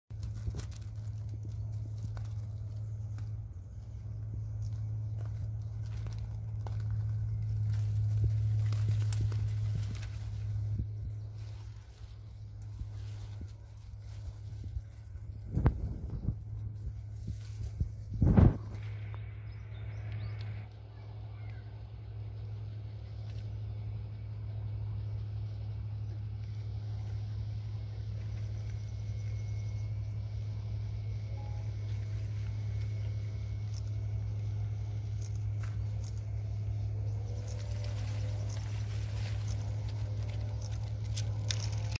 Yellow birdwings butterfly
Very rare. First time to see it here.